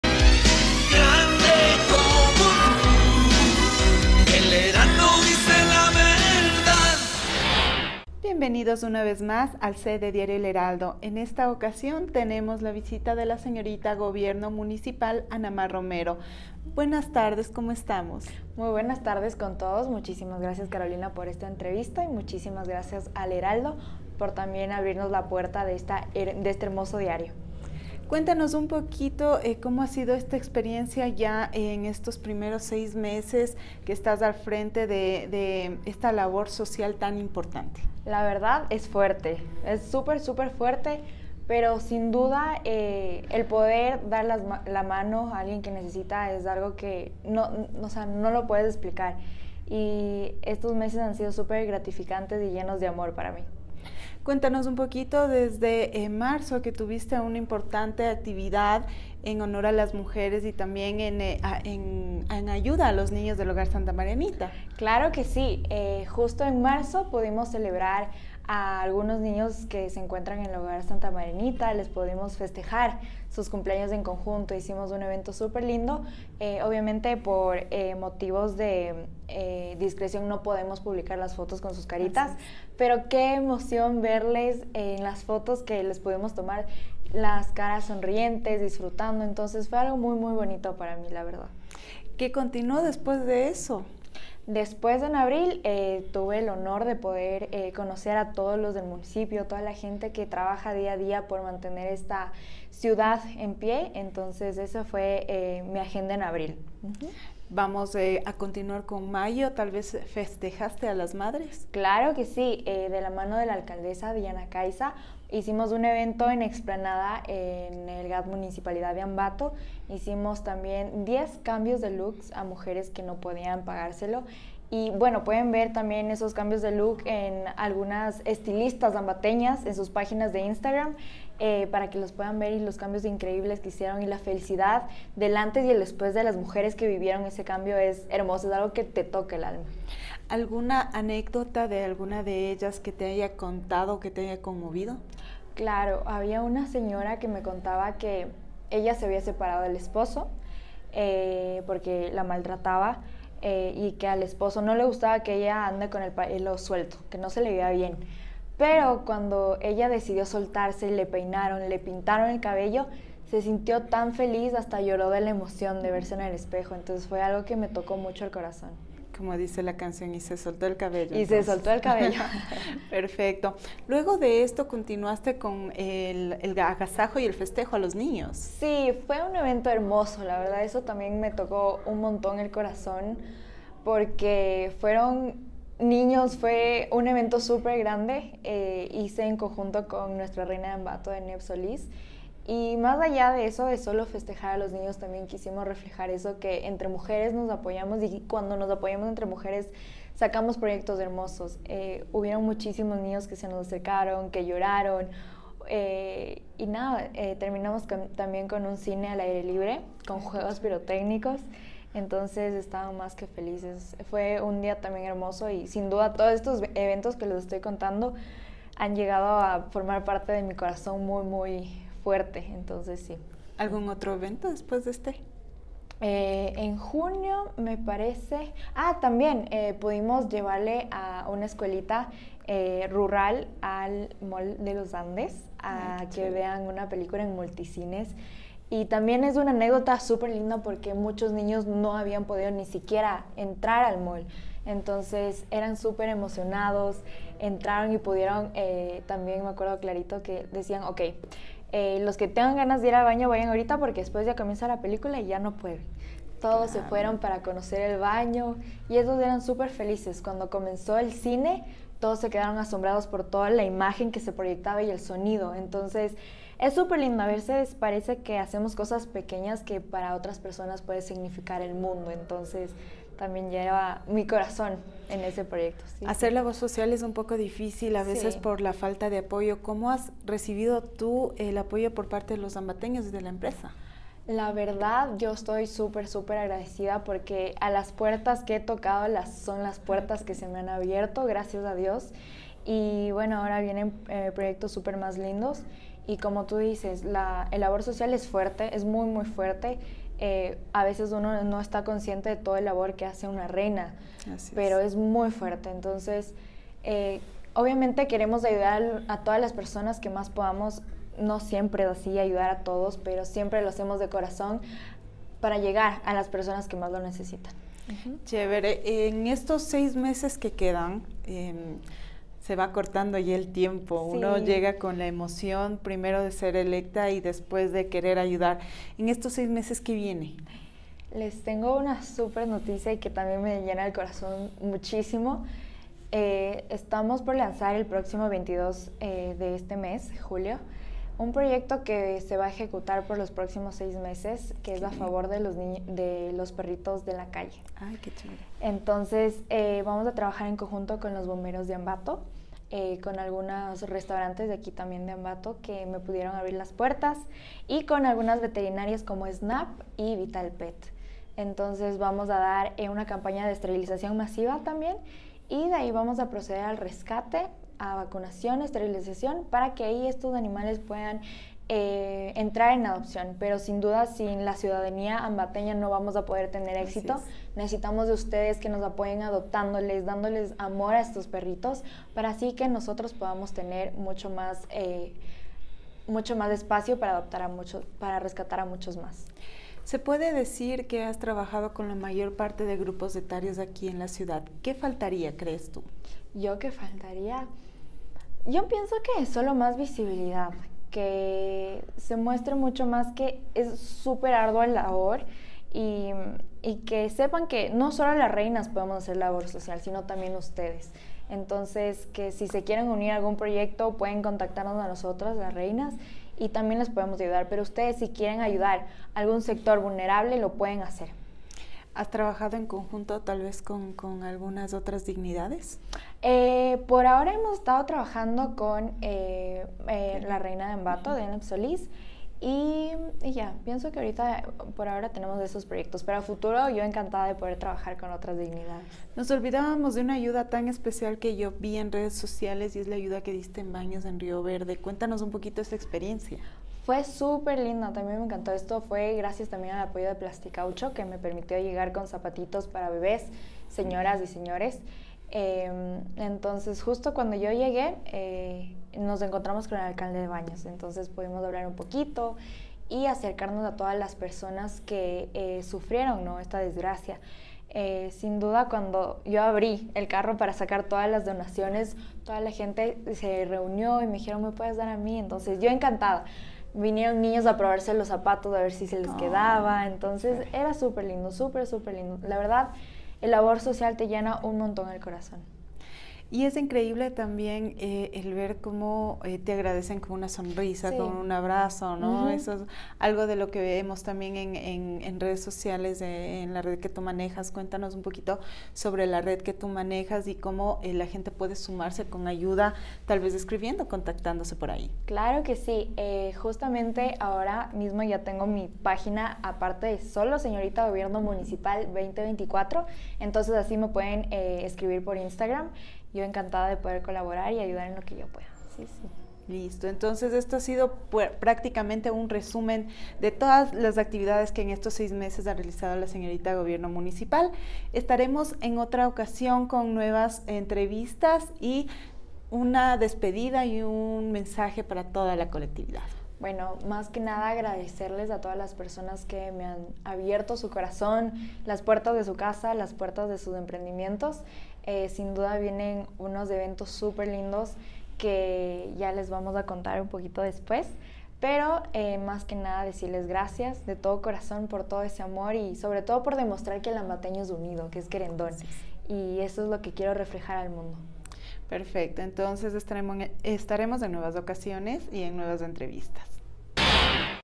▶️🎥 Anamar Romero, señorita Gobierno Municipal, promueve acciones sociales. (17-07-2024)
🎙️Entrevista | 🔴▶️🎥 Anamar Romero, señorita GobiernoMunicipal, en diálogo con nuestro medio de comunicación, detalla, hoy 17 de julio de 2024, los diferentes proyectos sociales que viene realizando en la ciudad y la ayuda que ha dado durante su gestión en este tiempo al frente de la dignidad a la que representa. Tungurahua Ambato 👇🏻